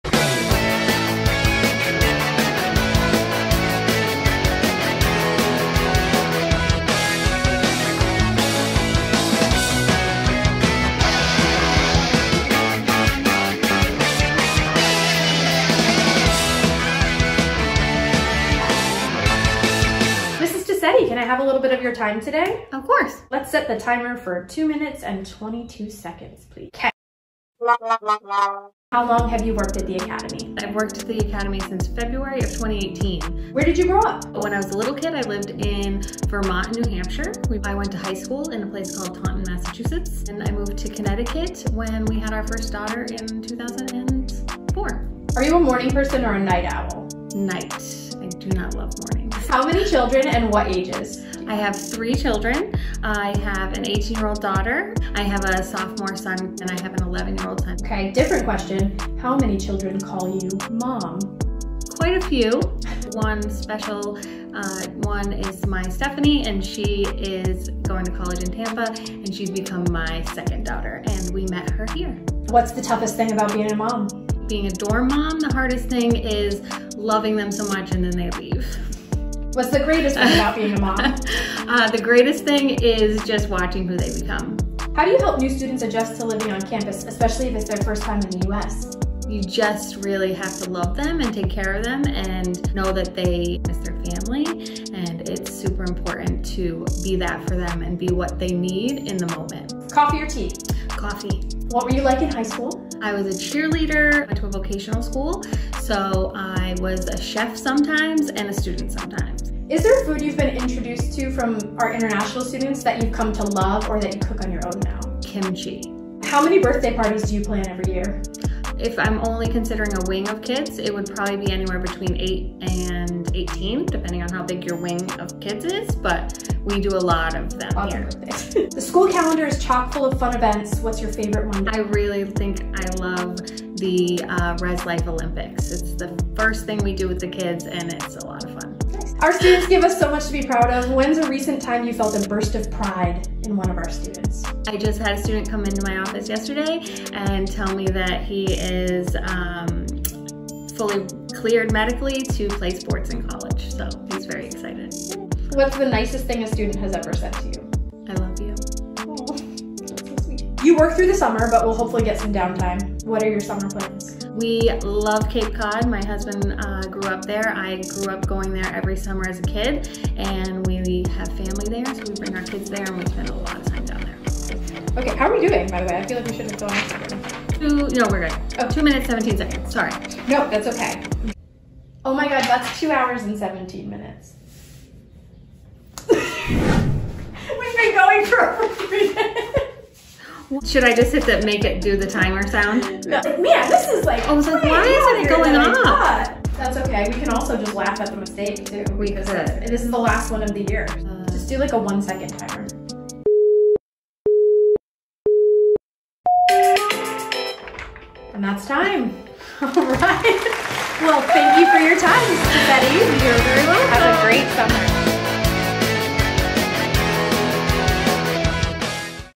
2:22 with Mrs. Amanda Tosetti
Meet our Director of Residential Life, Mrs. Tosetti. We had a laugh filming the final 2:22 video in this year's interview series! Watch till the end to find out why!